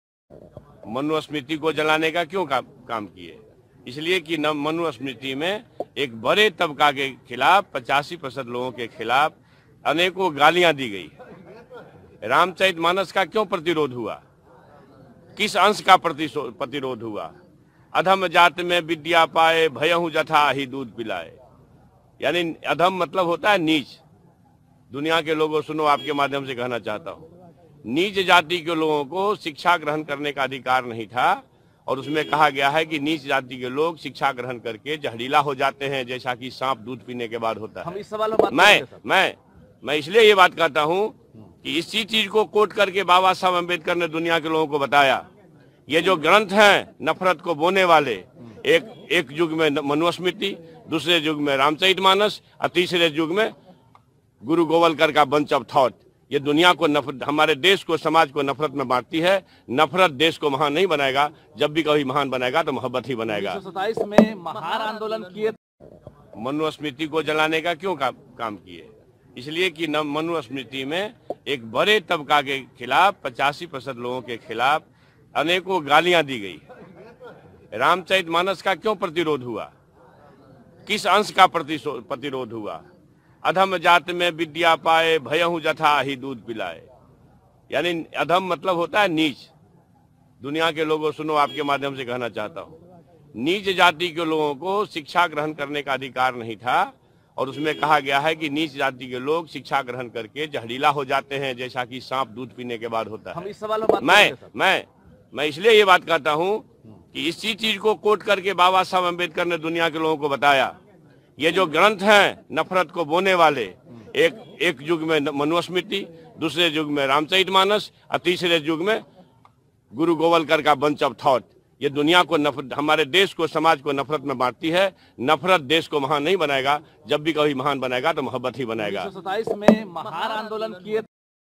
समाज को बांटता है Ramcharitmanas, Bihar के शिक्षा मंत्री Chandrashekhar का विवादीत बयान | Bihar News
Bihar के Education Minister ने कहा की  “श्री रामचरित मानस”  ग्रंथ दुनिया में  'नफ़रत'  फैलाने का काम करती है।